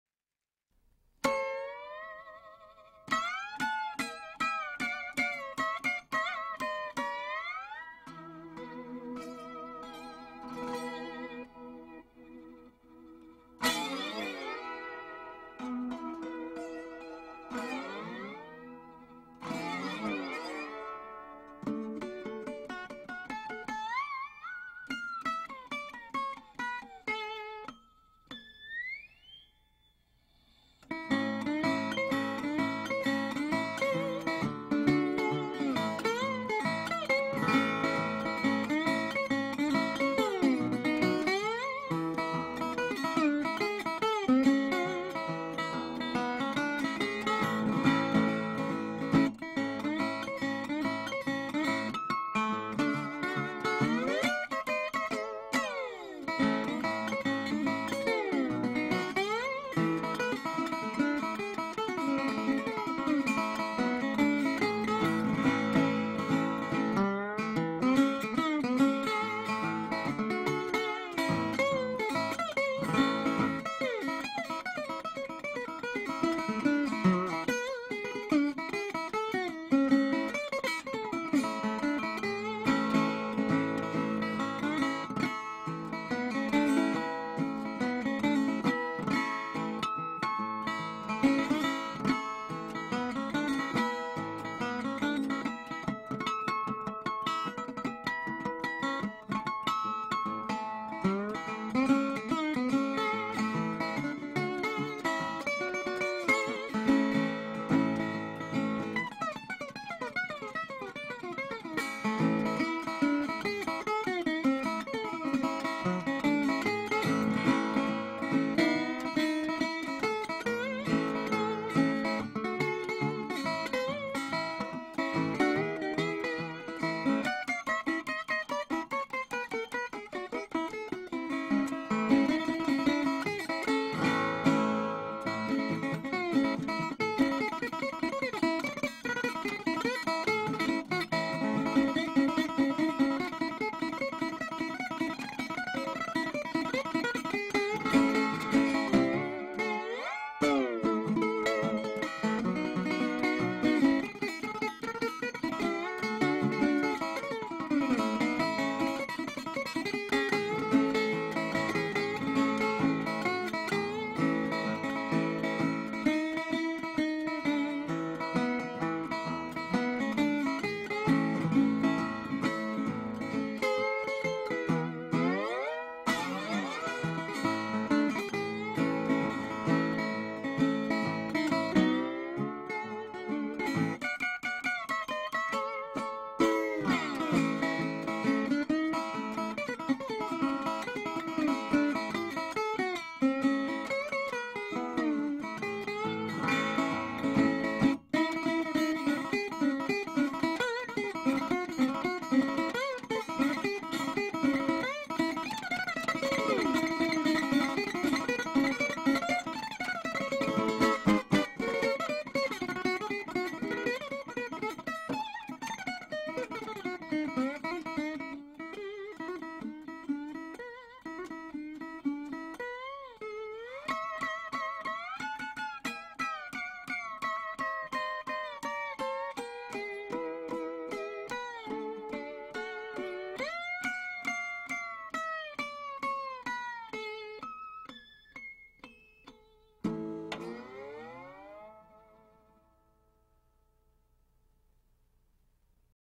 bob brozman fingerstyle
bob brozman on the dobro slide. i dont recognize the tune